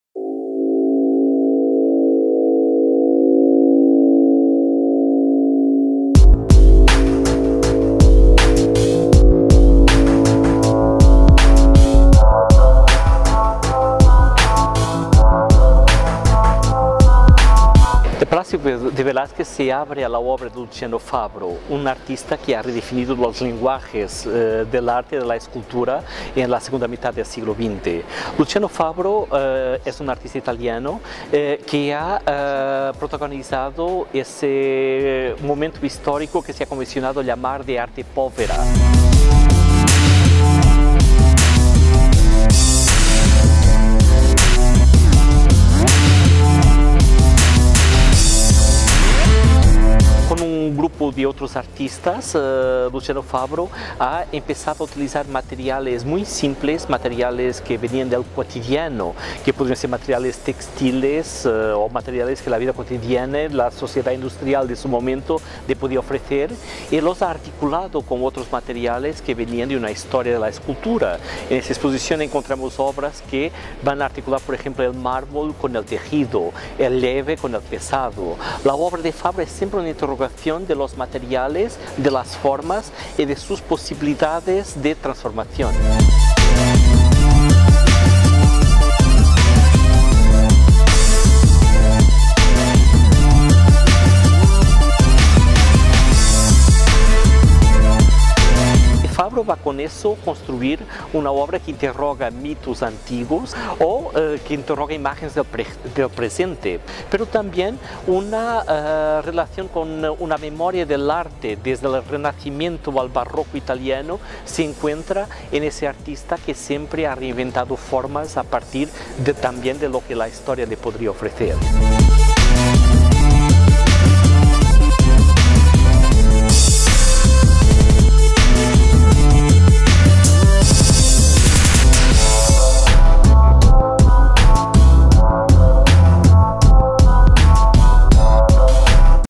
Luciano Fabro
La obra del artista italiano Luciano Fabro (Turín, 1936 - Milán, 2007), uno de los principales representantes del arte povera, se expone en el Palacio de Velázquez hasta el próximo mes de abril. La muestra antológica hace un recorrido por los 40 años de su trabajo, a través de más de 50 obras realizadas en mármol, seda, espejo, cristal de Murano, bronce, latón, pelo de reno, pasta italiana y acetato, entre otros materiales.